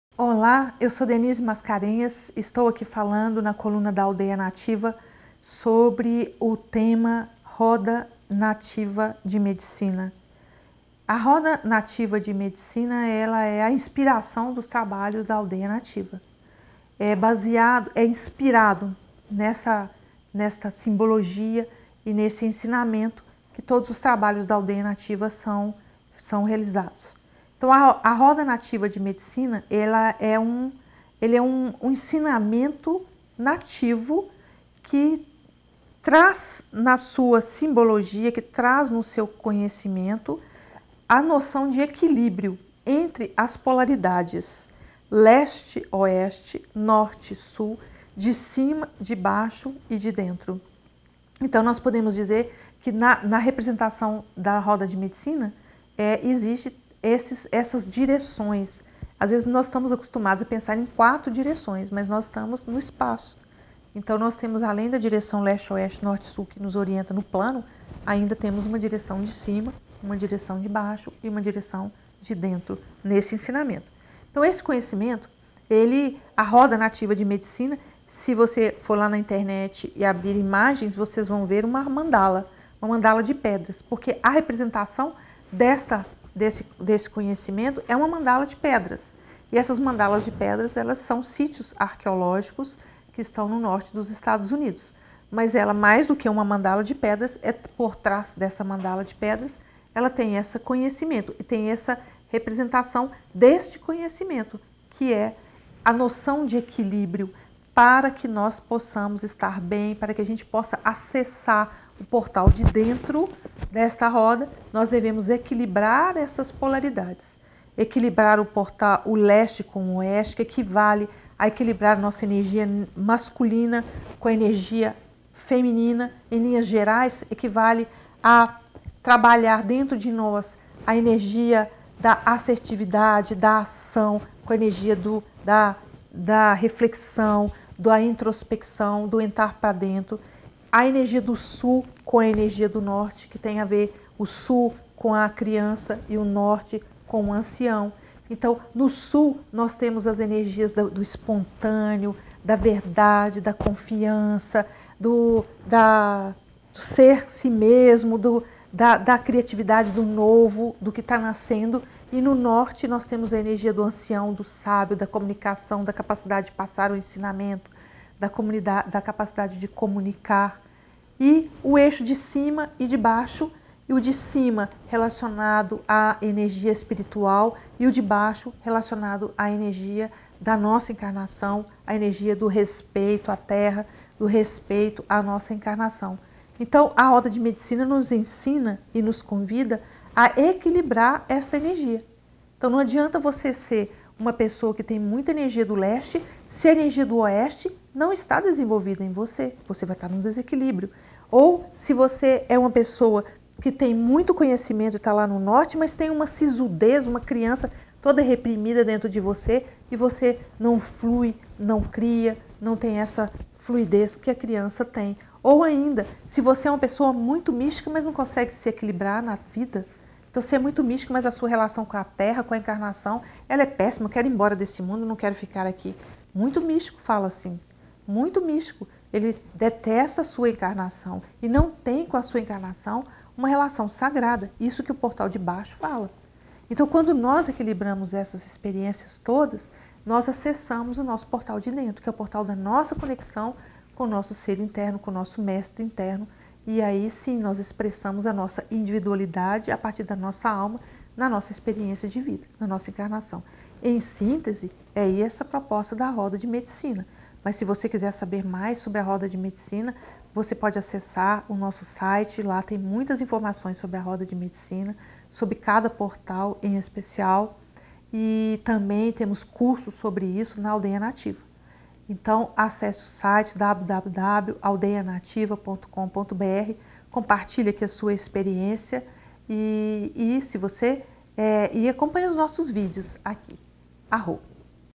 O que é a Roda de Medicina? - Denise Mascarenhas [Aldeia Nativa #08]
Coluna: Aldeia Nativa
Tema: O que é a Roda de Medicina?
Direção/Edição: Samuel Souza de Paula

COMPARTILHE e deixe seus comentários!!!

TEMAS ANTERIORES...